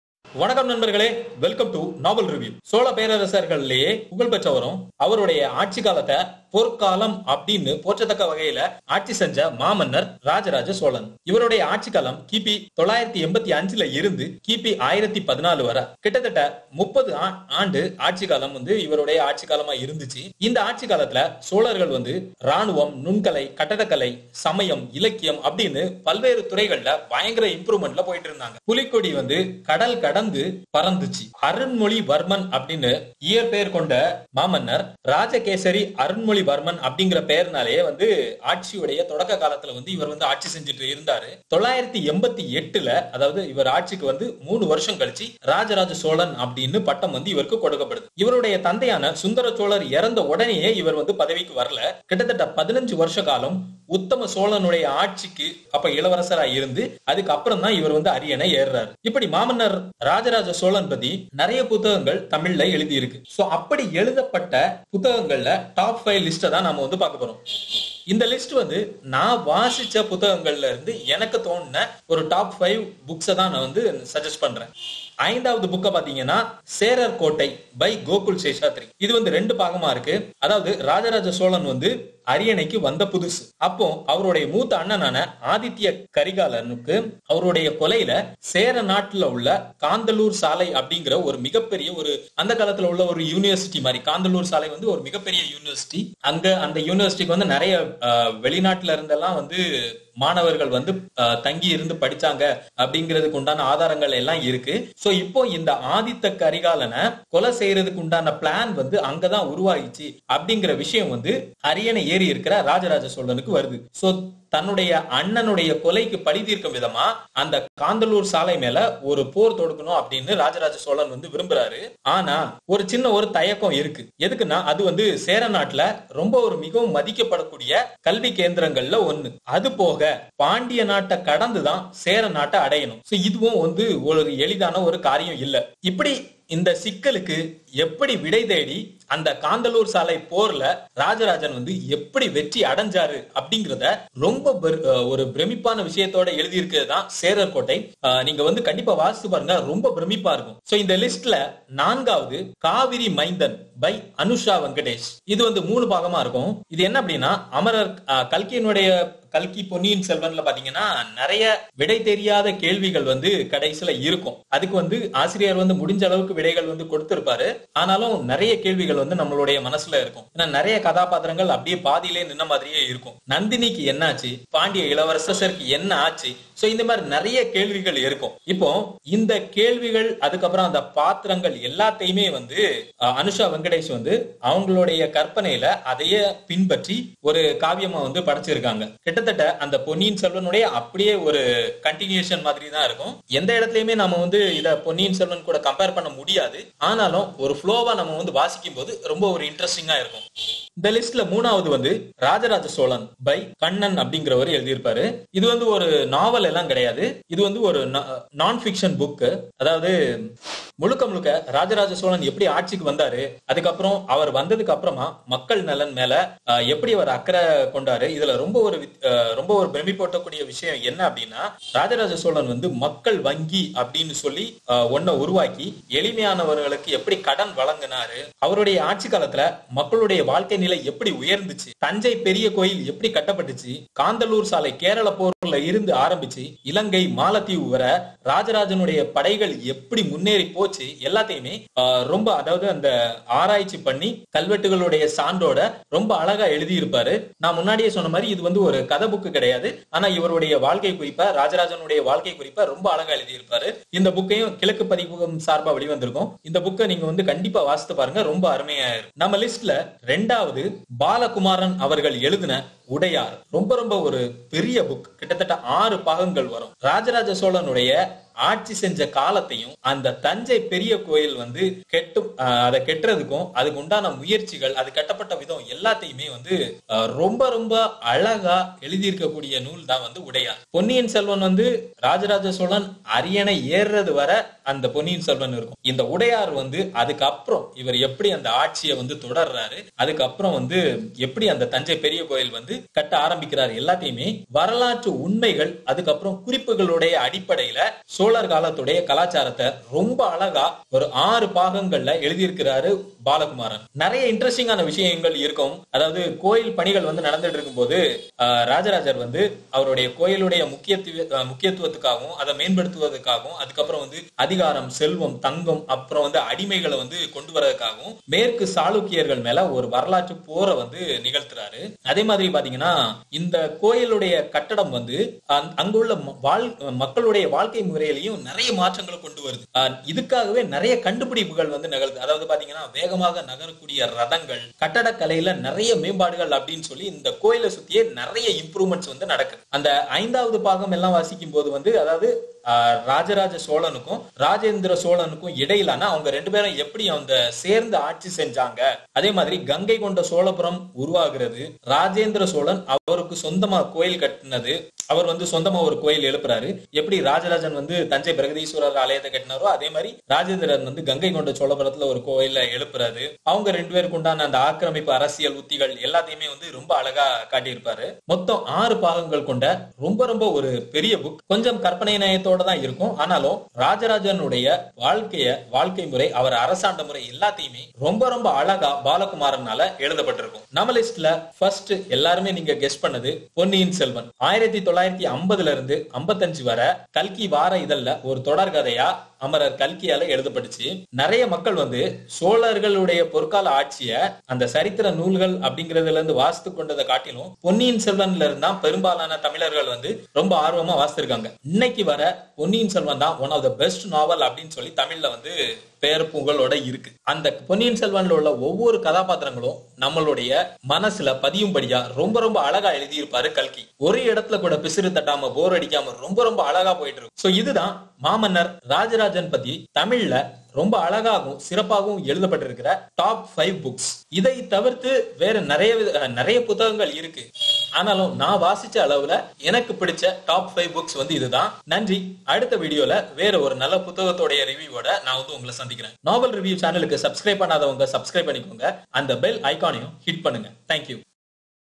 Raja Raja Cholan : Top 5 Books | Novel Review
Novel Review presents the top 5 books on Raja Raja Cholan. From Kalki's Ponniyin Selvan to Gokul Sheshadri's Cherar Kottai. Please find the top 5 books

Novel Review: Tamil's first Book Tube channel. Find summary, information & review of books in Tamil. தமிழின் முதல் Book Tube channel, Novel Review.